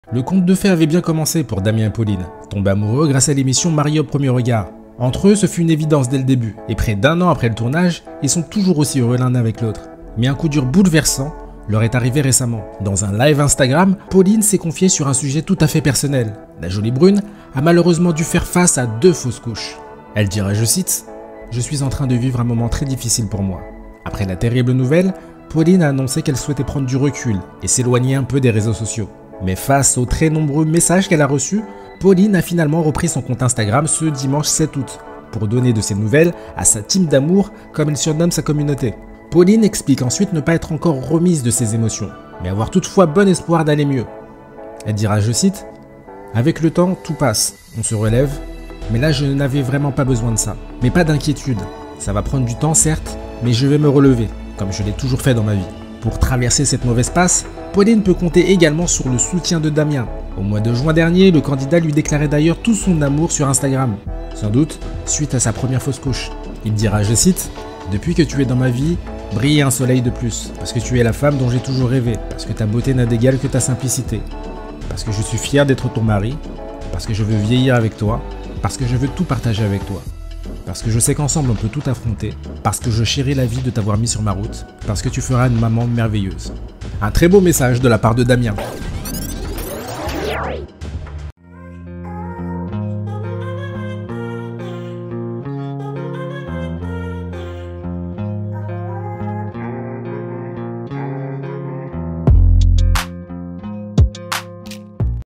Pauline (MAPR 6) fait une déchirante révélation sur Instagram
Sur Instagram , Pauline Tancredi, une des candidates de la dernière saison de "Mariés au premier regard" a fait une triste annonce à ses abonnés. Enceinte de deux mois, la jeune femme qui avait épousé Damien devant les caméras de la chaîne M6, a une nouvelle fois perdu son bébé...